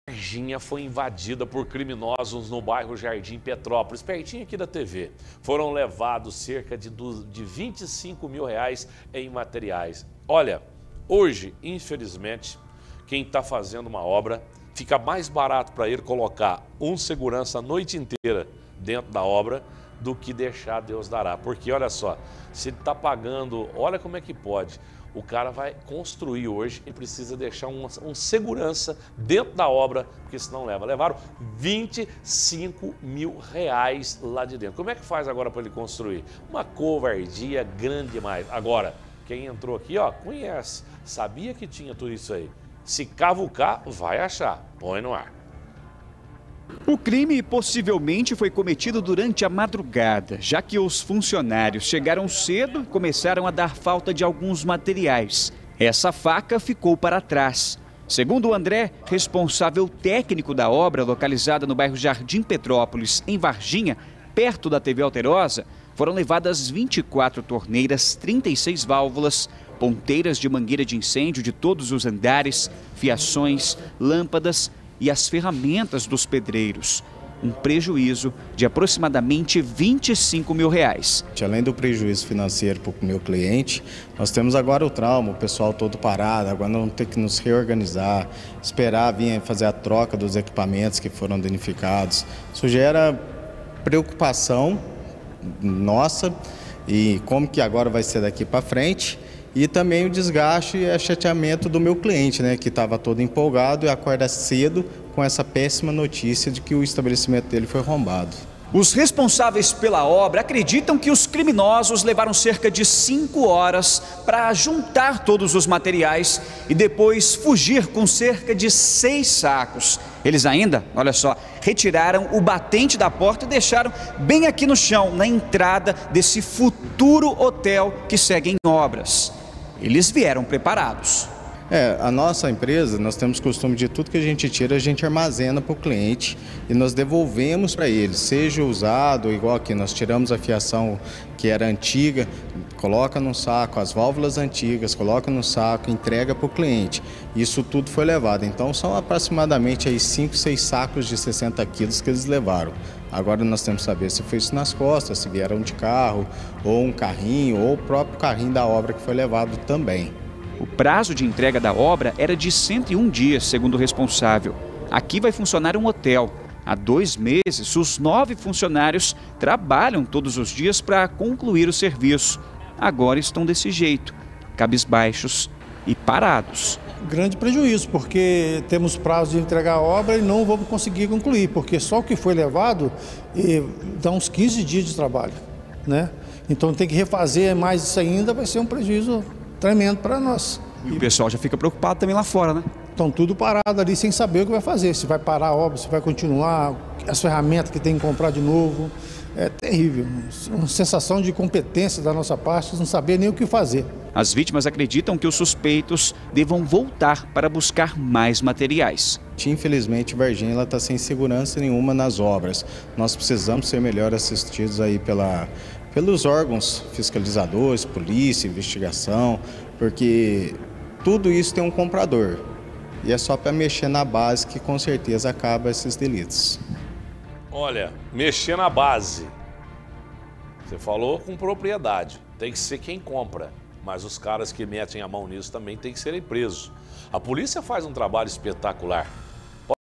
Obra é invadida em Varginha
OBRA É INVADIDA: PREJUÍZO DE R$ 25 MIL | Uma obra foi invadida no bairro Jardim Petrópolis em Varginha. O prejuízo gira em torno dos R$ 25 mil.